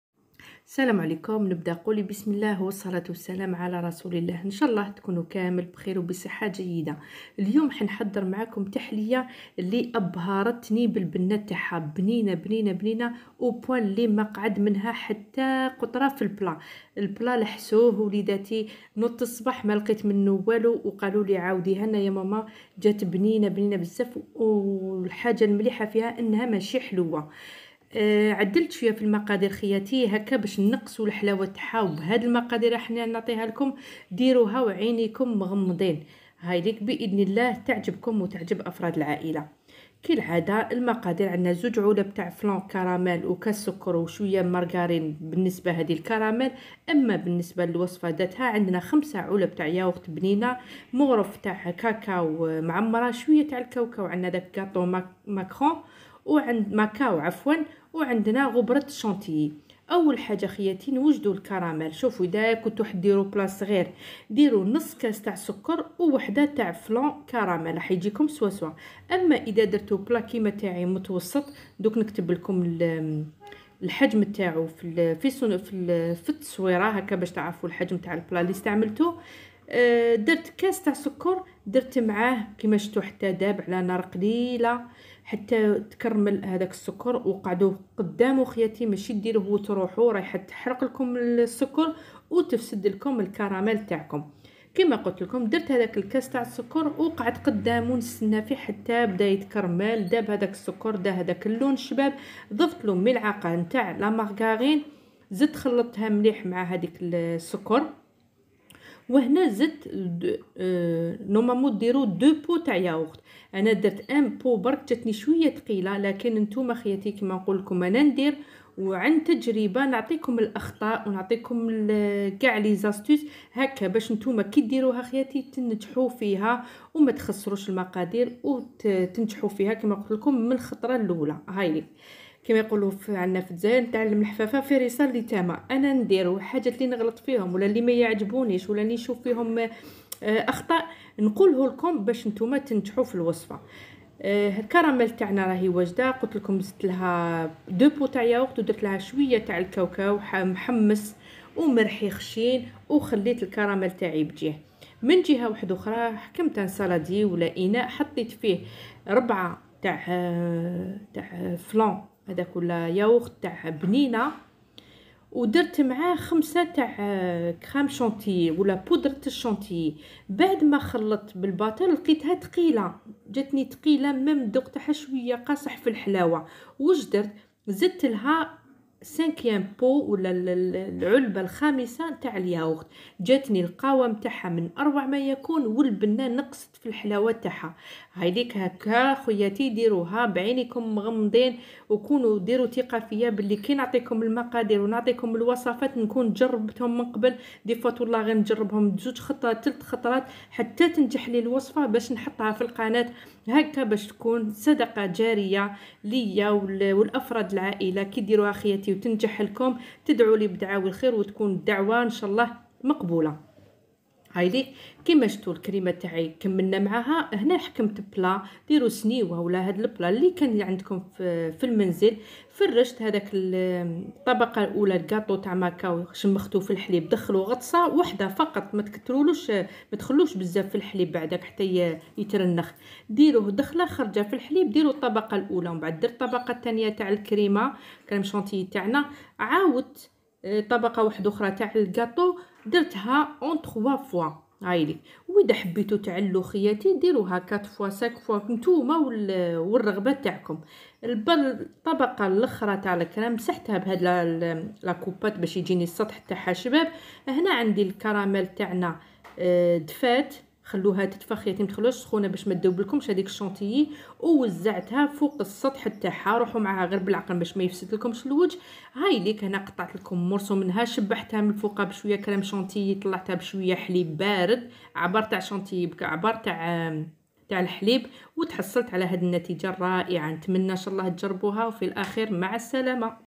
تحلية الأحلام🛌السعيدة 😇مقادير قليلة 🤏او البنة كبييييرة👍جربوها اكيد😊 رايحين تعاودوها❤
سلام عليكم ورحمة الله وبركاته انشاء الله ينال الفيديو اعجابكم
لا تنسو زر الاعجاب و الاشتراك والضغط على الجرس ليصلكم كل جديد
المقادير:
5ياغورت بنينة
4نفس العلبة  تاع الياغورت بودرة الشونتيي
ملعقة كاكاو
بسكوت ماكاو
بالنسبة للكراميل
كاس سكر
2علب فلون كراميل
ملعقة كاوكاو محمص او مرحي خشن